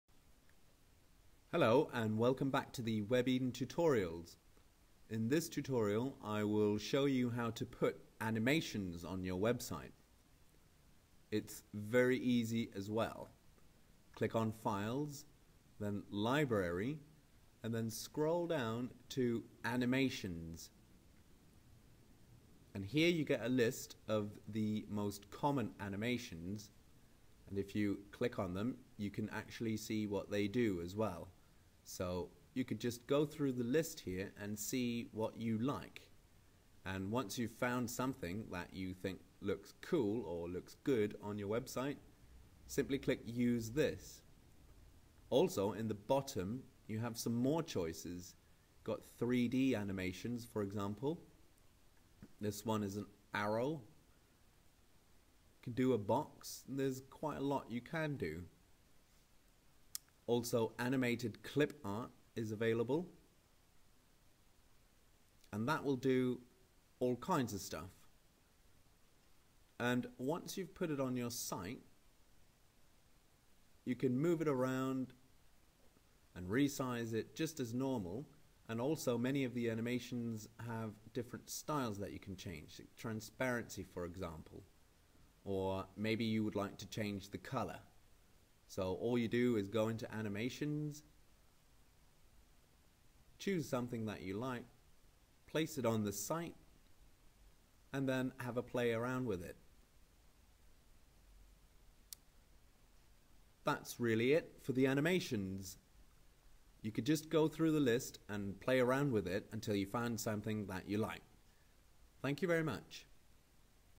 Adding Animations: WebEden.co.uk Free Website Builder
Webeden is an online website building service. This is a tutorial showing you how to add animations to your